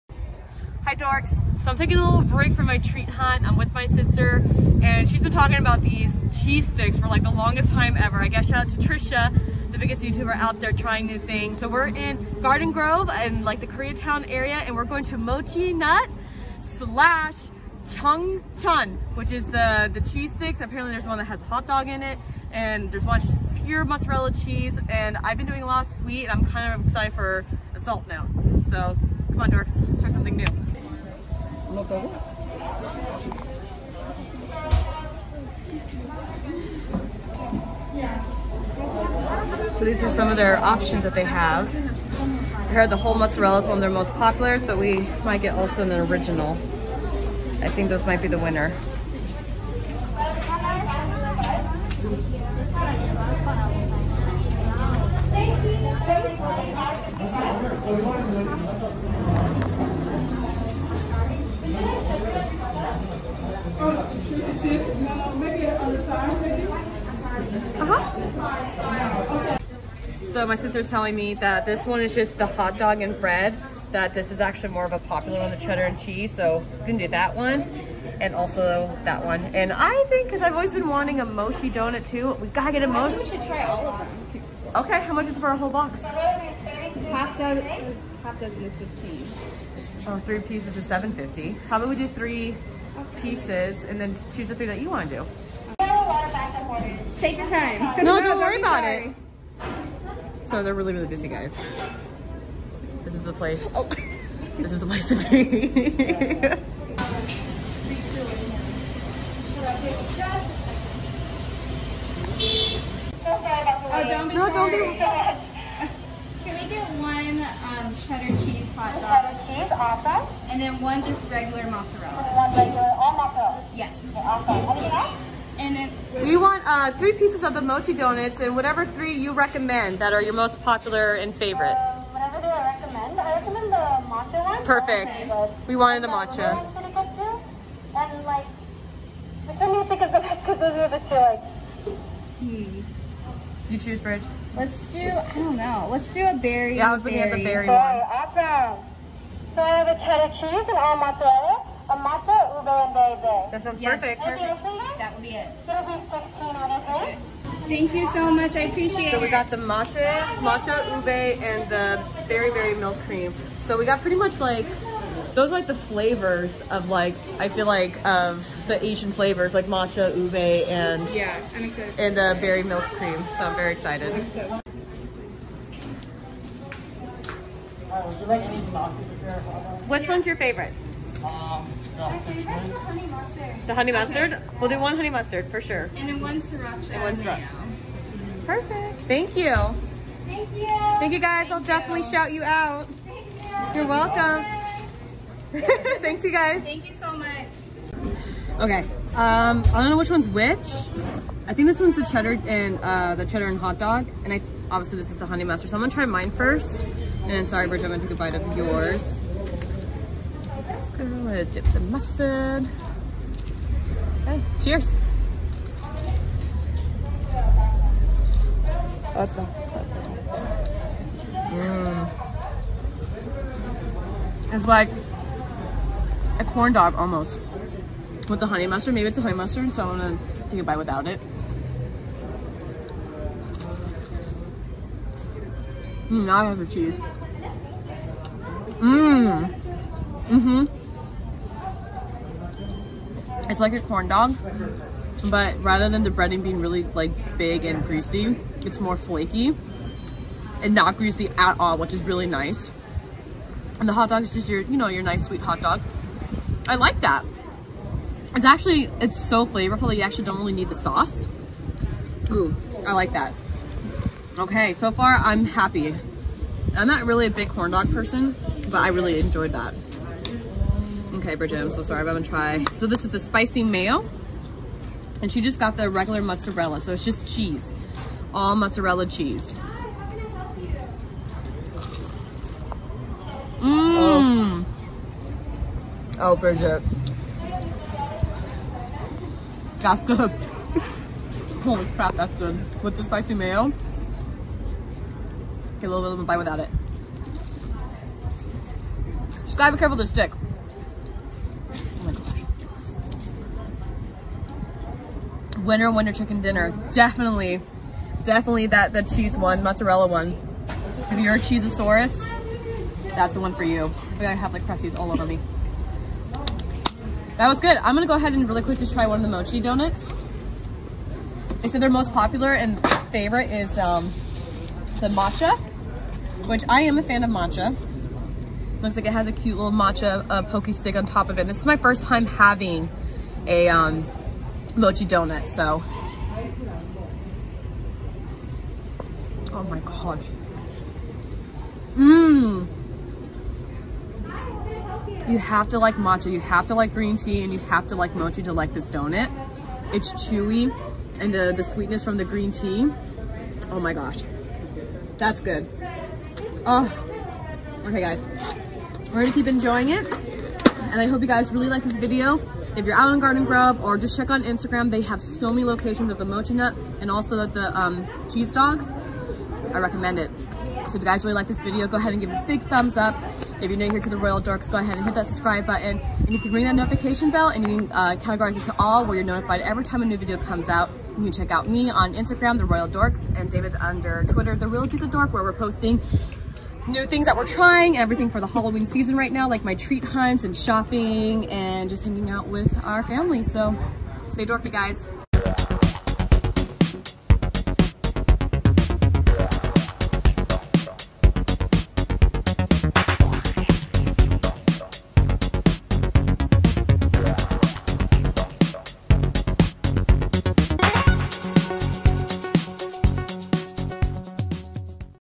Trying a Chung Chun hot dog for the first time!
Liz took her camera to Garden Grove to try a Chung Chun hot dog.  Something we've been hearing about for quite a while, and finally she's going to try one!  It's kind of like a corndog, but covered in rice instead of batter, then fried. some have cheese as well, while others are JUST CHEESE!!

You can also follow our weekly Disneyland adventures on YouTube at Fresh Baked and Stuff